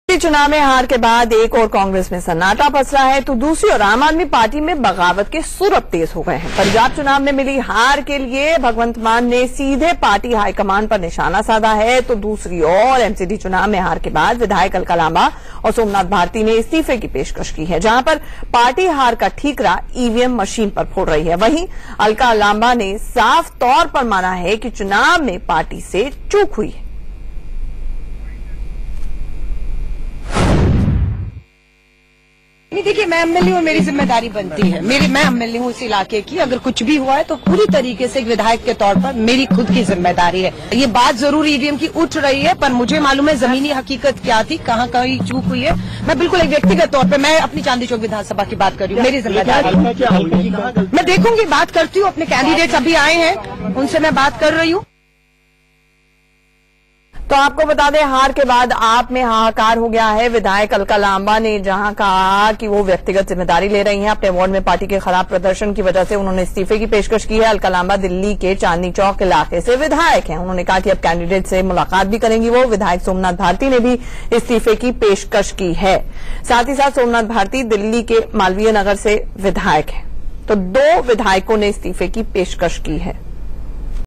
MCD results: Bhagwant Mann slams AAP leadership; Alka Lamba & Somnath Bharti to tender resignation
Bhagwant Mann slams AAP leadership.

Aam Aadmi Party (AAP) MLA Alka Lamba on Wednesday offered to resign from all party posts taking the moral responsibility for the party's defeat in the municipal seats falling under her constituency.

"I offer to quit from all the party posts and MLA post taking the moral responsibility for the defeat of the party in all the three wards of my constituency," Lamba, who represents Chandni Chowk constituency in central Delhi tweeted.